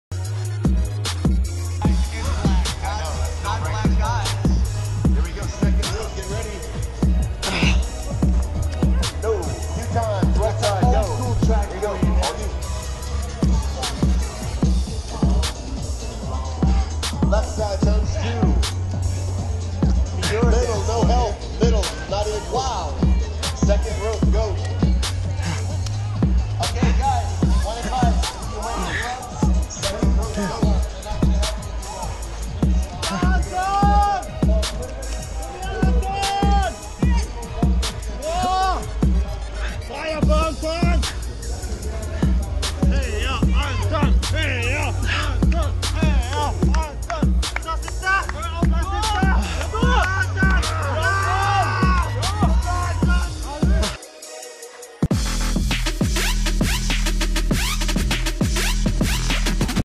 Best of Toughest 5 سباق القدرة بعيون مغربي - السويد
Full race Toughest Gothenburg 2017 - Sweden
Toughest Göteborg 2017 - Sverige
سباق القدرة Toughest بعيون مغربي مشارك في السباق في مدينة غوتنبورغ السويدية